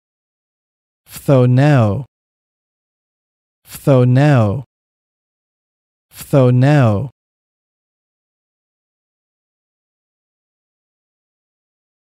How to pronounce Phthoneō in Biblical Greek - (φθονέω / be envious)
φθονέω (phthoneō) means be envious in Biblical Greek.

This video shows how to pronounce φθονέω in the Erasmian pronunciation system.

Example Bible Verses:

Galatians 5:26
We must not become conceited, provoking one another, envying one another.